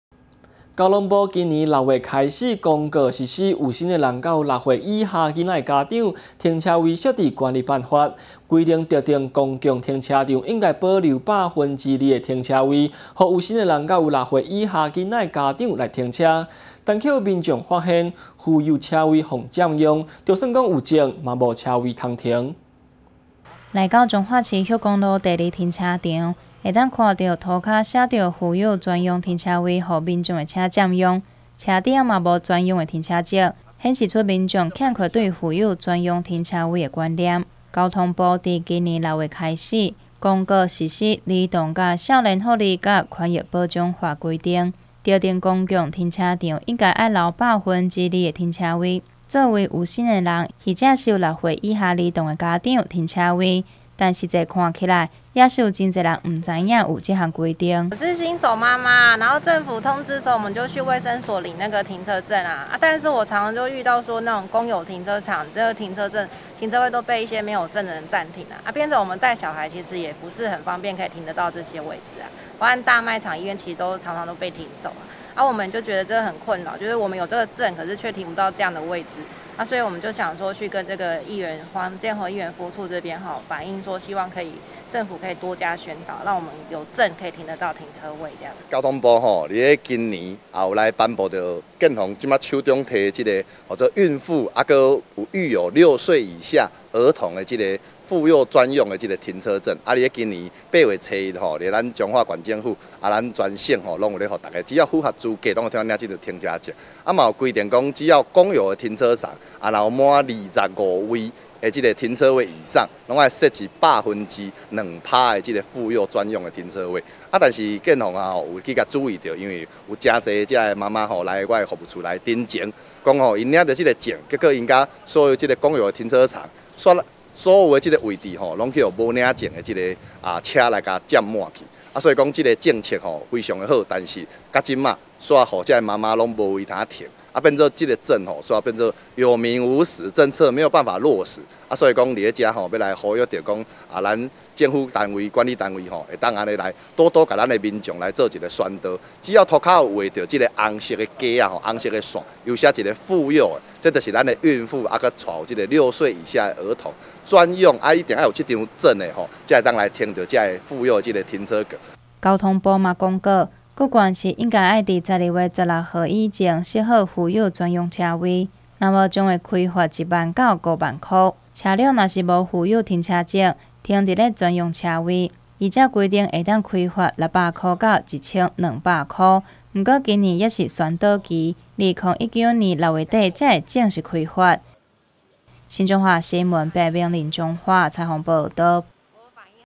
新彰化新聞20181031 婦幼車位遭占用 新手媽媽怨有證無車位可停
交通部今年六月起公告實施「孕婦及育有六歲以下兒童者停車位設置管理辦法」，規定特定公共停車場應保留百分之二的停車位，作為孕婦及育有六歲以下兒童者的停車位，但卻有民眾發現，婦幼車位被占用，就算有證也沒車位可停。